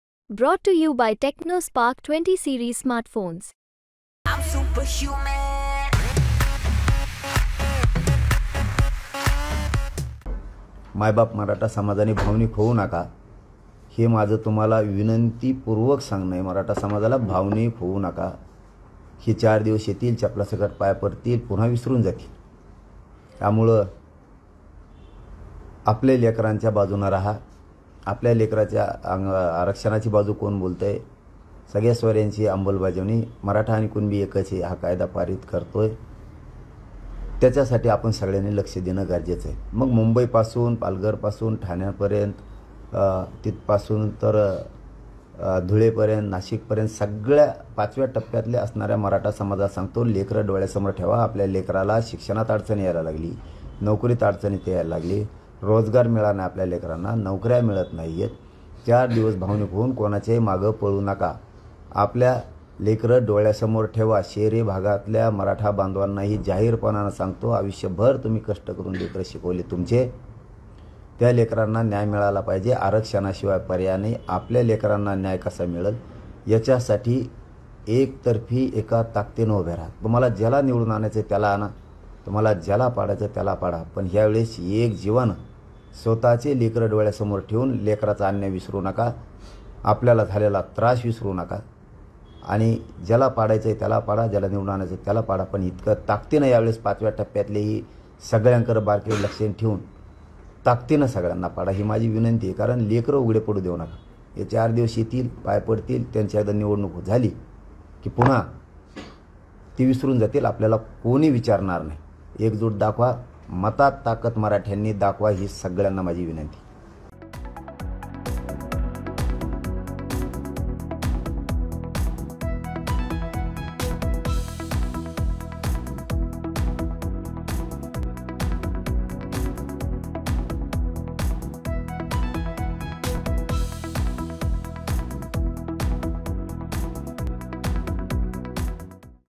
Manoj Jarange Patil यांचं मराठा समाजाला भावनिक आवाहन, काय म्हणाले? | Lok Sabha Election 2024
मनोज जरांगे पाटील यांचं मराठा समाजाला भावनिक आवाहान, म्हणाले, ताकदींनं सगळ्यांना पाडा